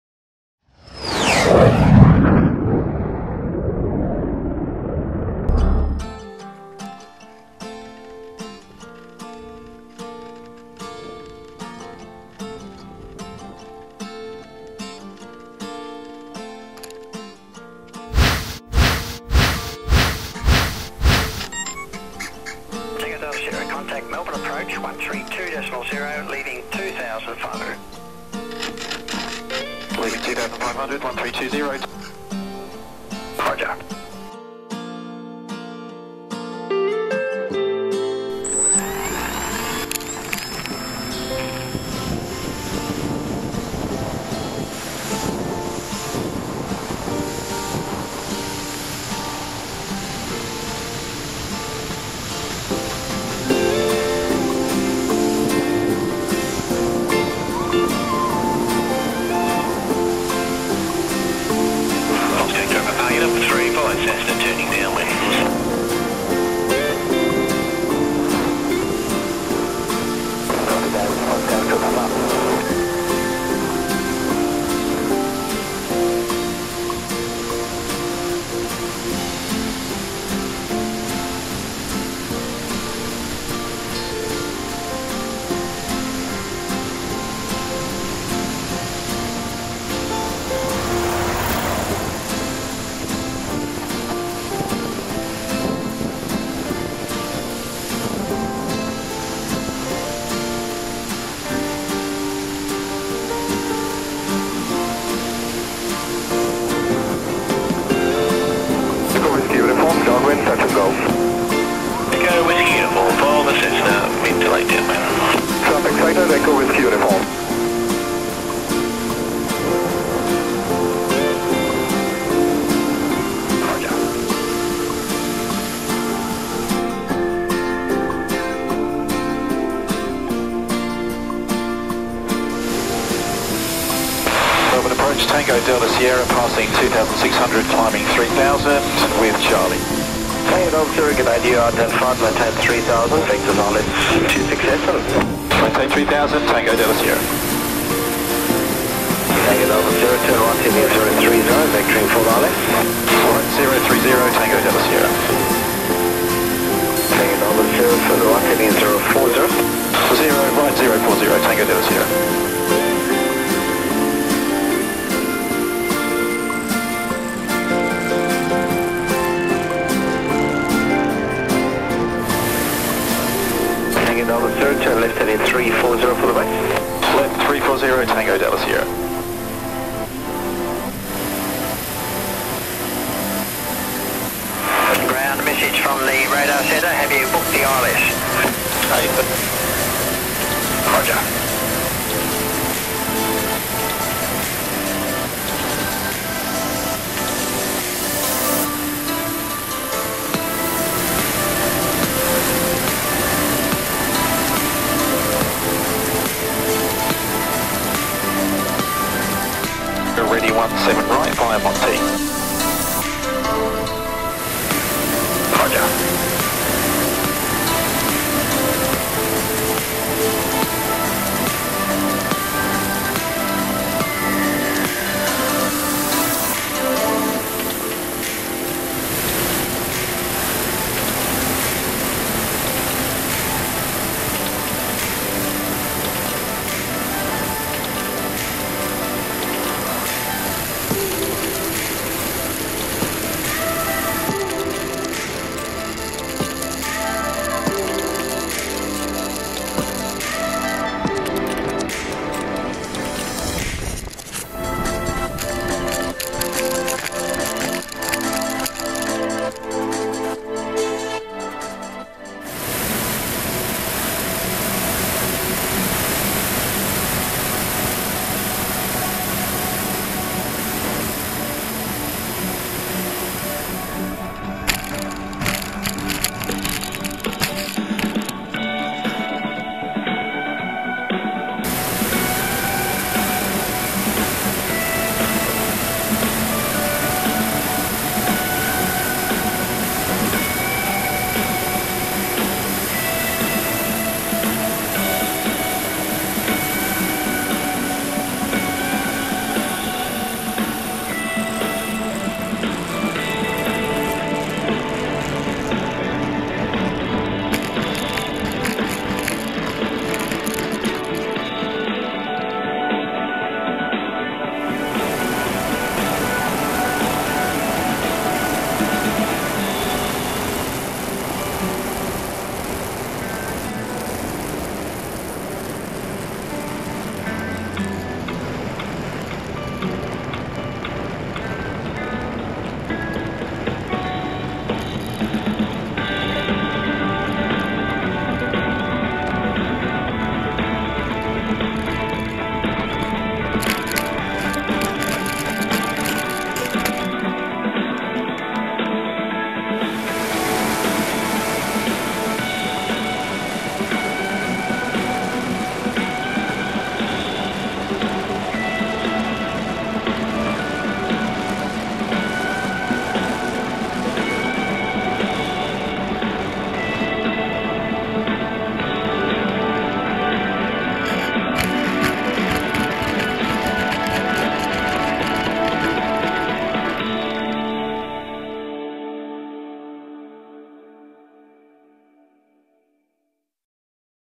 E-Flite Turbo Timber Autumn March Coastal Multi Cam Flights
Timber Turbo over coastal Victoria Australia March 2020 Fall season